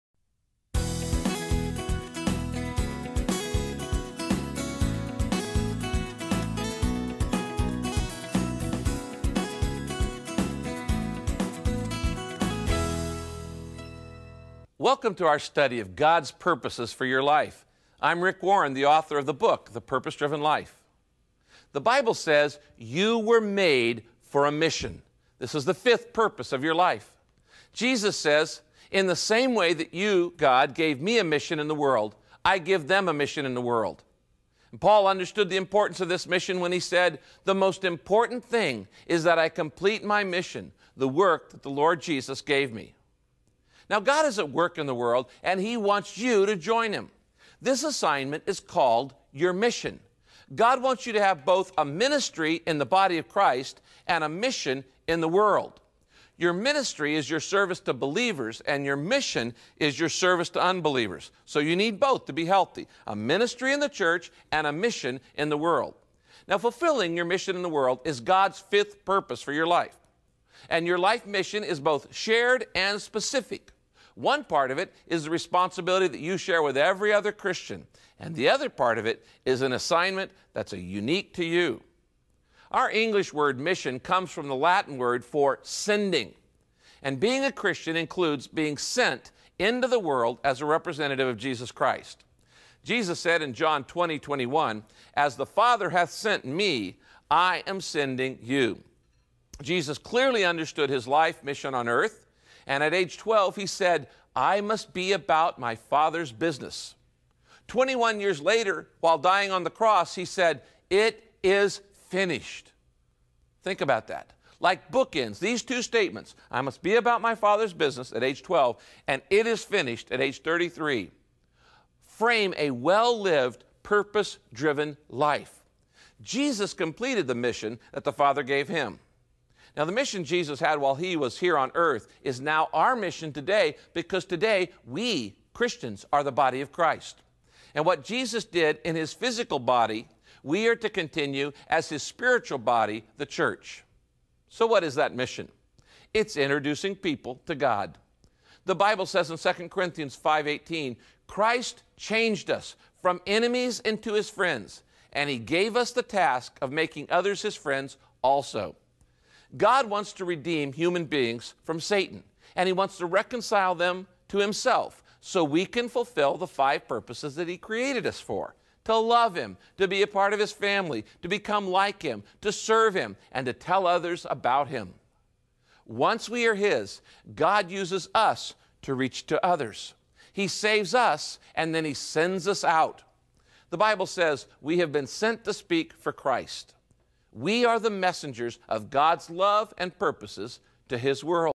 The Purpose Driven Life: Made for a Mission - Video Curriculum, Vol. 6
This DVD accompanies the Volume 6 - Made for a Mission study guide. This study will help you discover how to live a life that is fueled and fulfilled by God's purposes. Each session features a video teaching by Pastor Rick Warren and Study Guides packed with thought-provoking questions and discussion prompts that will engage participants in thoughtful study, application, and prayer. This series is great for small groups or adult Sunday school. Choose the purpose you want to study, or take them in order.